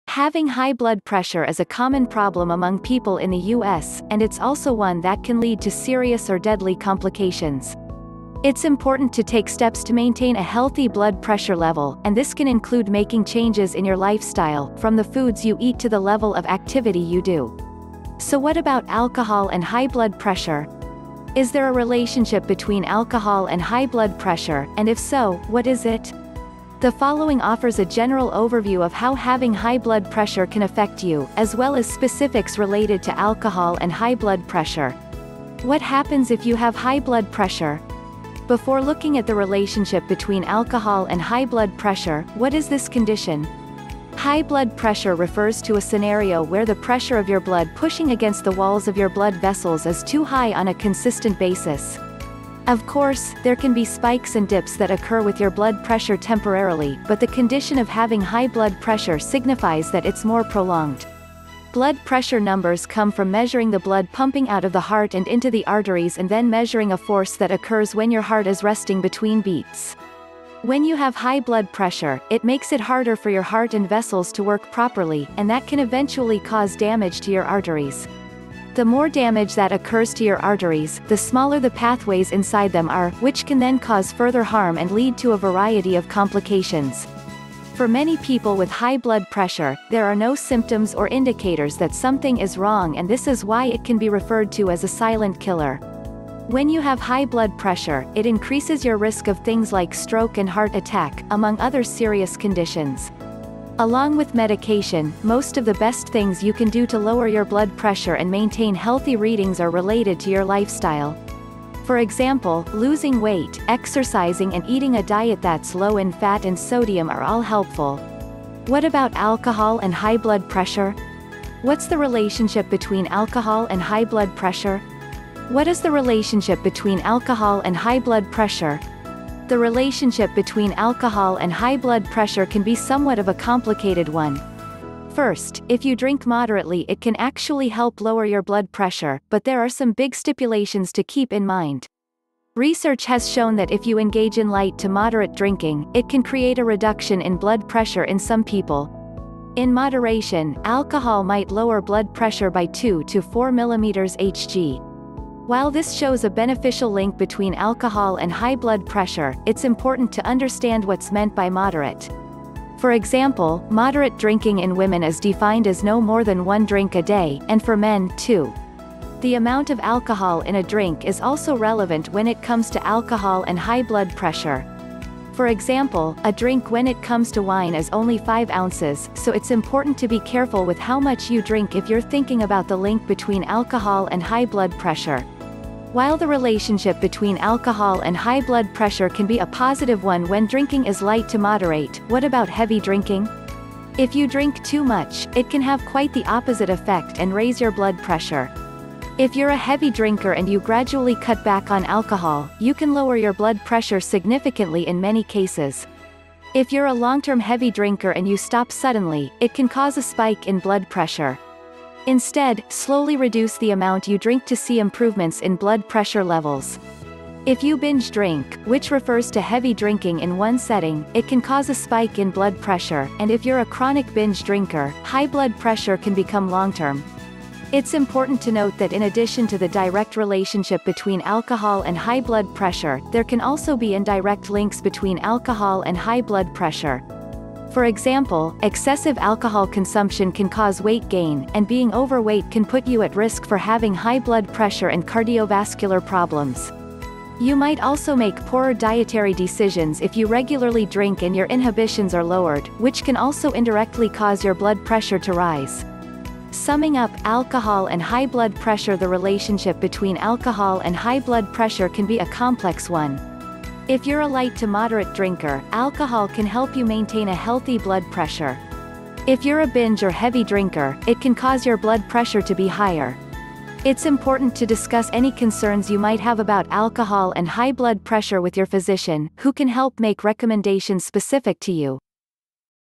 Alcohol And High Blood Pressure - The Relationship Between Alcohol and High Blood Pressure
Having high blood pressure is a common problem among people in the U.S., and it’s also one that can lead to serious or deadly complications. It’s important to take steps to maintain a healthy blood pressure level, and this can include making changes in your lifestyle, from the foods you eat to the level of activity you do.